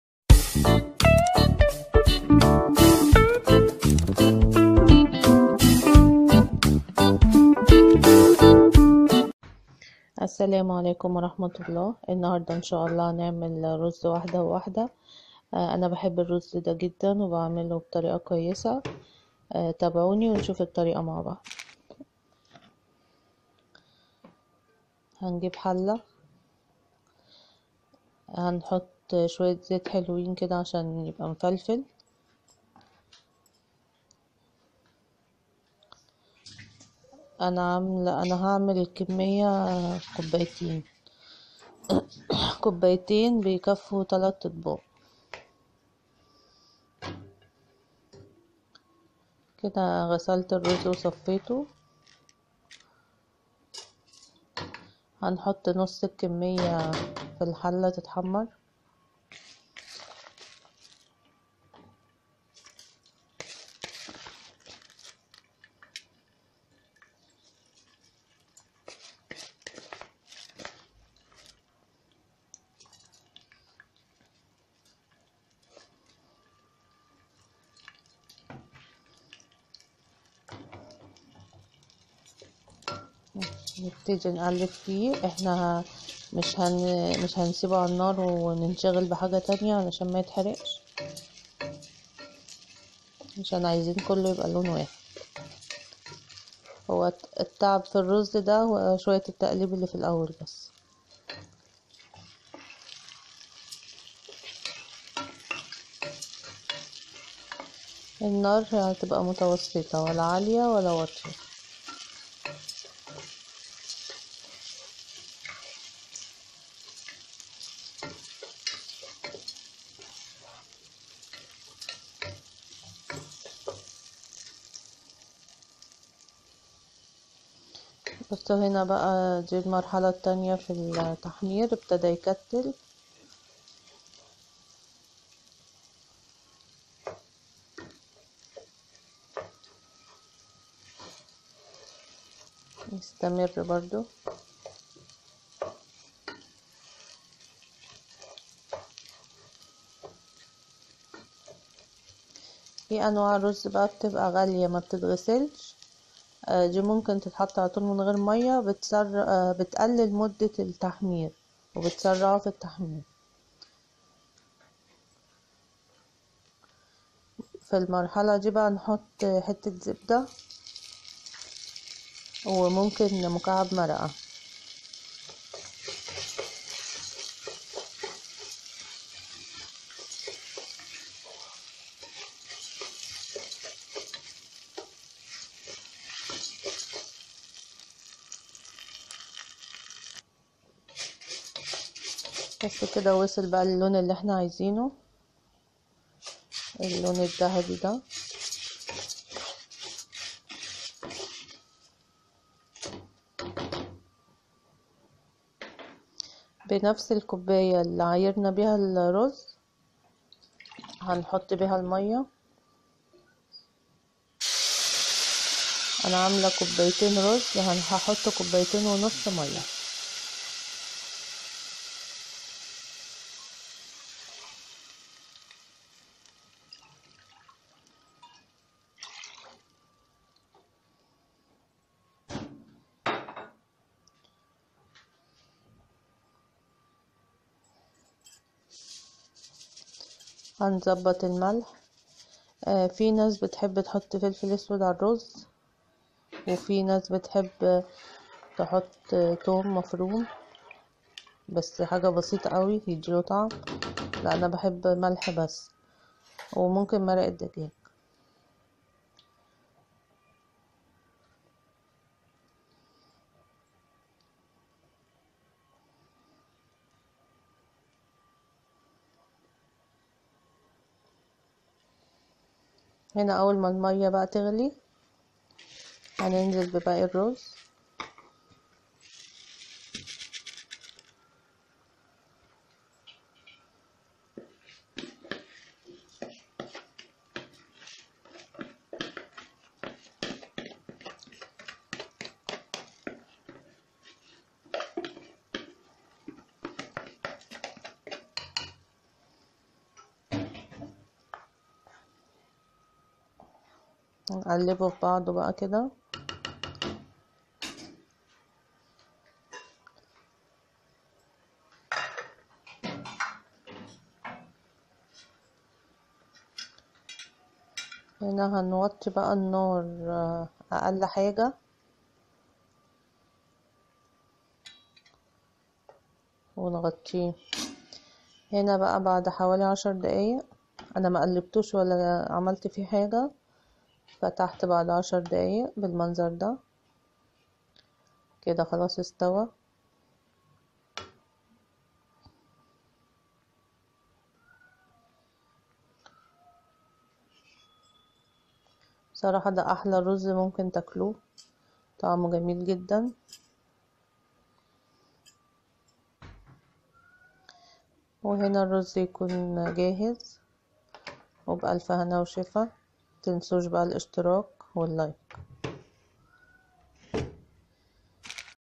احلى رز ممكن تاكلوه فى حياتكوا ( أرز حبه وحبه )
أرز-حبه-وحبه
طريقة عمل أرز حبه وحبه بطريقة سهله جدا